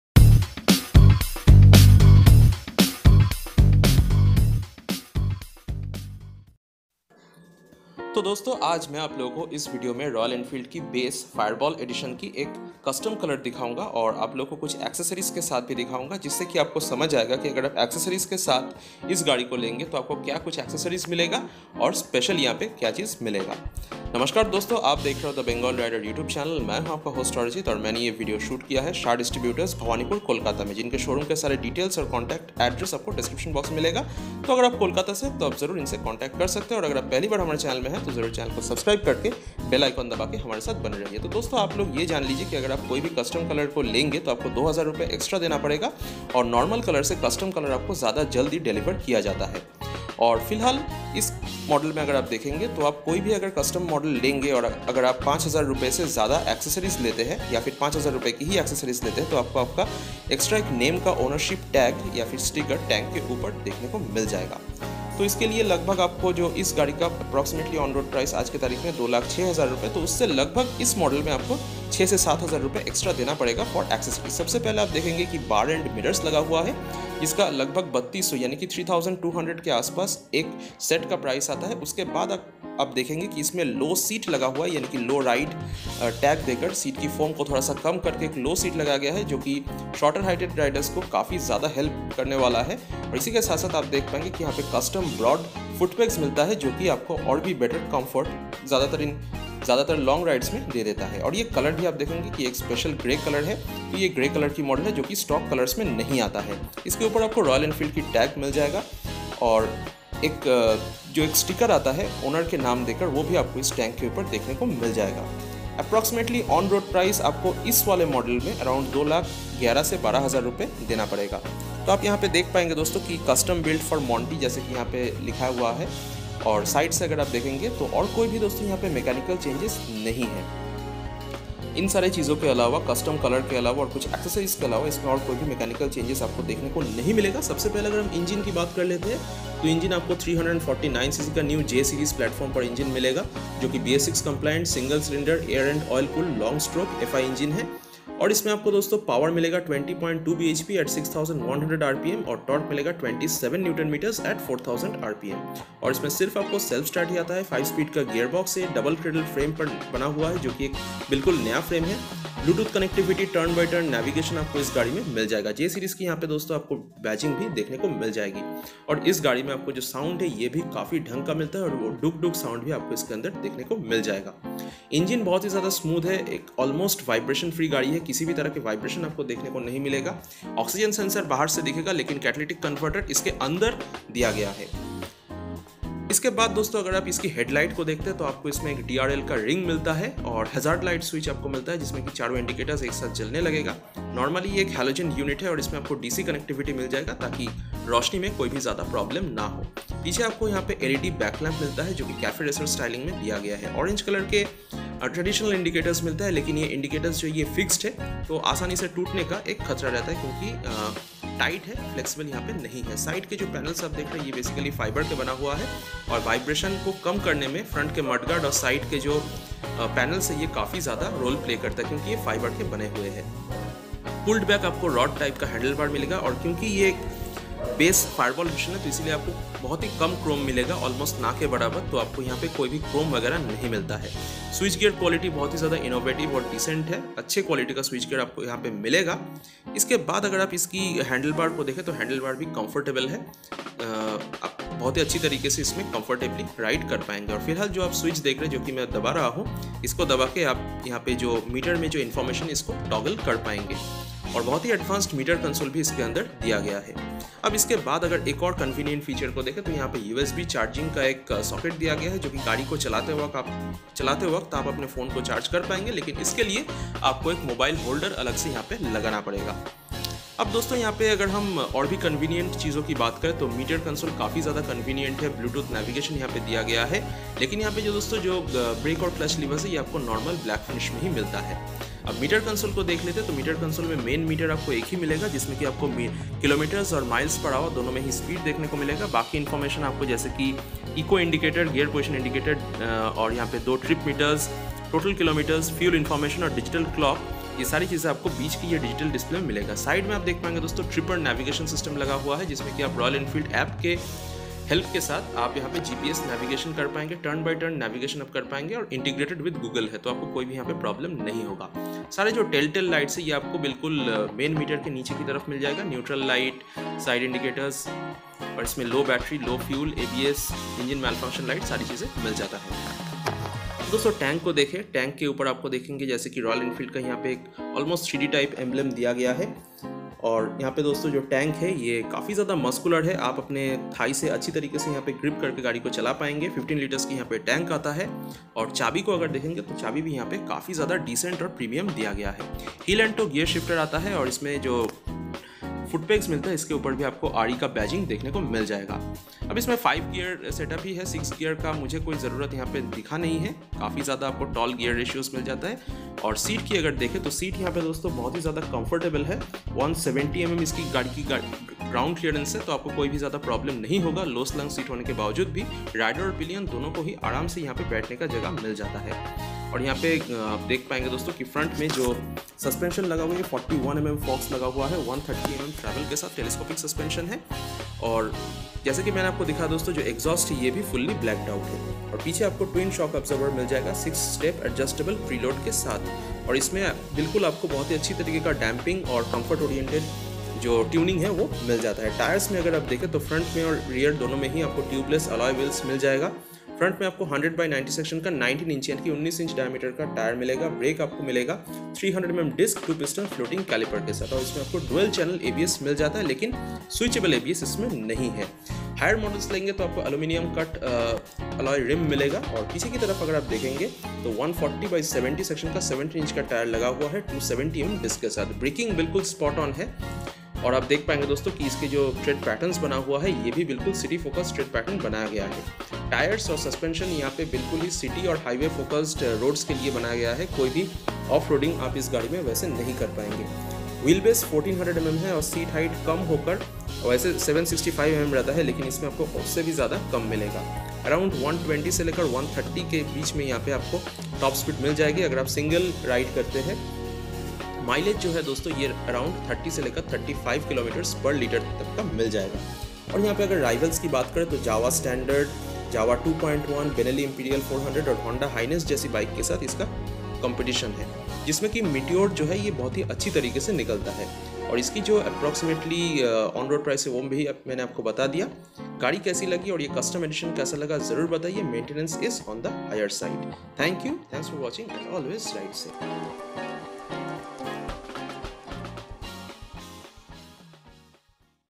Royal Enfield Meteor 350 Fireball Custom Model with Accessories | Detailed Review
RE Meteor 350 Fireball edition custom grey with accessories!

Courtesy - Shah Distributors (Royal Enfield Dealer)
Address - 8/1 Sarat Bose Road, Bhowanipore, Kolkata